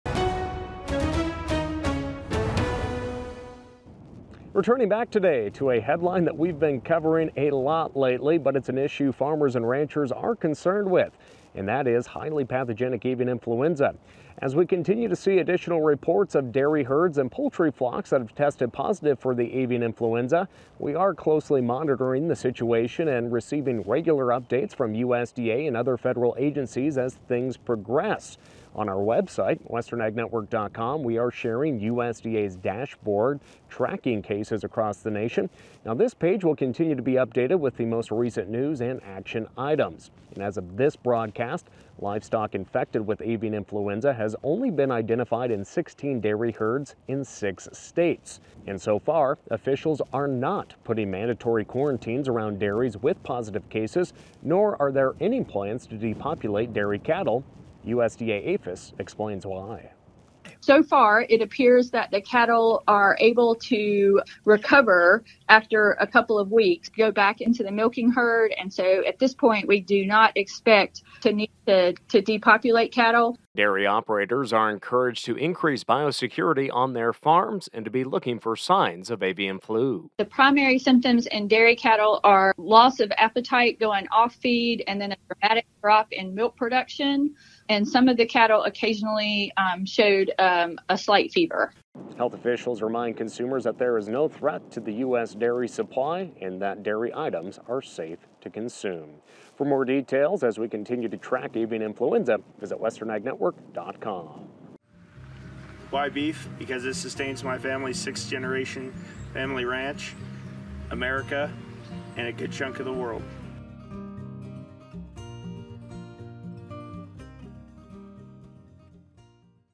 Confirmed Cases of HPAI in Domestic Livestock
As cases of Avian Flu appear in dairy cattle in parts of the nation, Western Ag Network has created a dashboard for producers to keep track of infections in livestock.